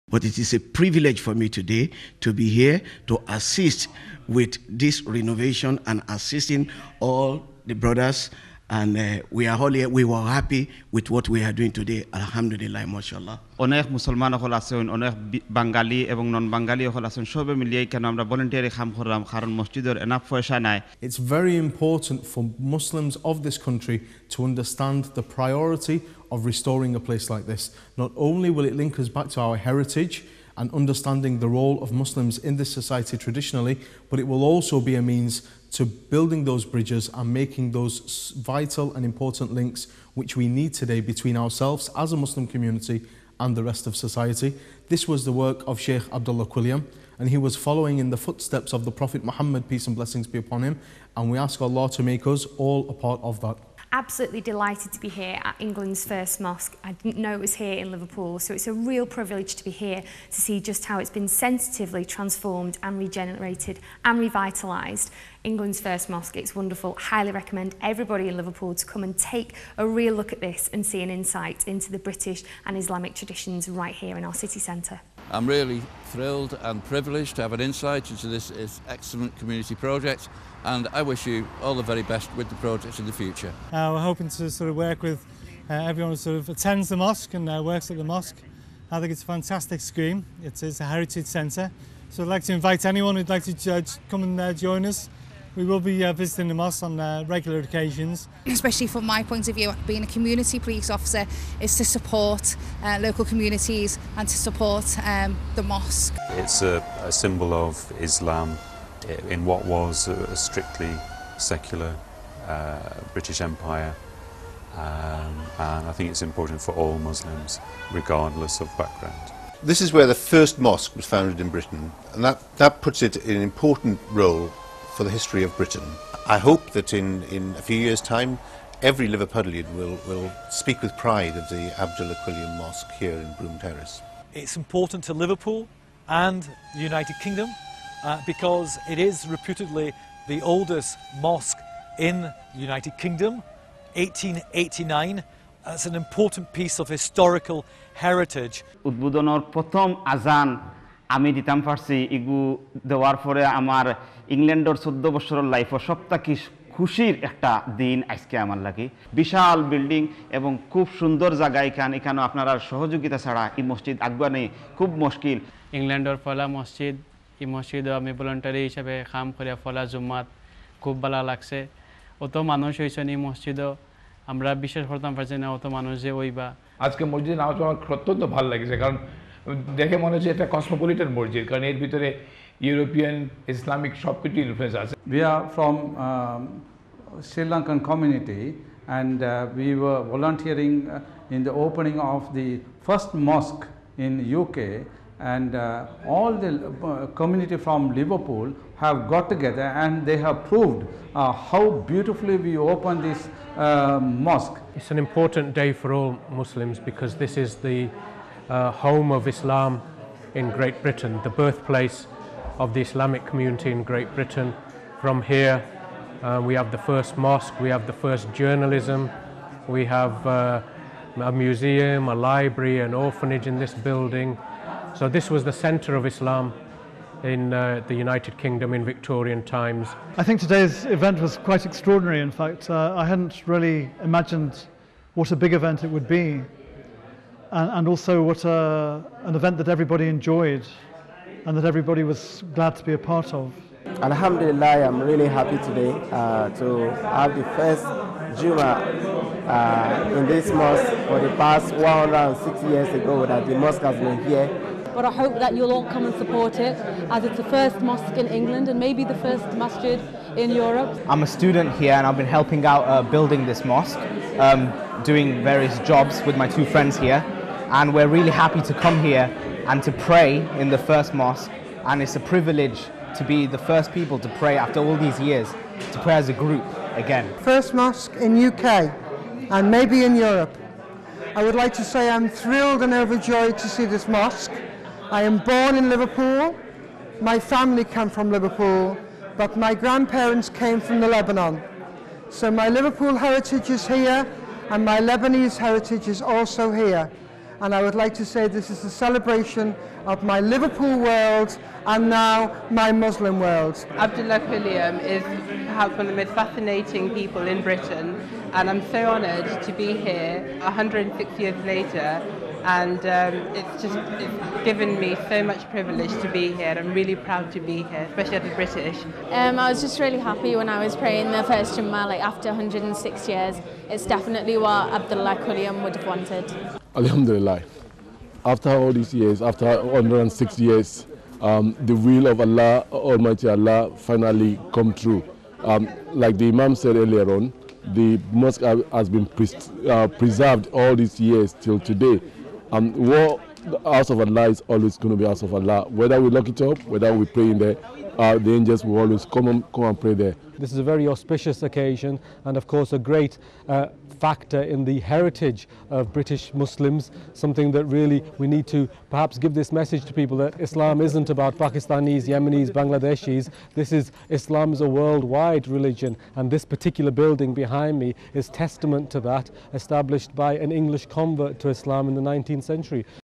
Abdullah Quilliam Mosque 2
England first Mosque established by English Reverted Muslim Abdullah Quilliam in 1889 at Liverpool, which has been refurbished and re-opened in the Holly month of Ramadan 2014 by Liverpool's Muslim community.
William Henry Quilliam, a local Liverpool solicitor and resident embraced Islam in 1887 (aged 31), after returning from a visit to Morocco, and took on the name Abdullah. He claimed that he was the first native Englishman to embrace Islam. His conversion led to a remarkable story of the growth of Islam in Great Britain. This history is now beginning to emerge and has important lessons for Muslims in Britain and around the world.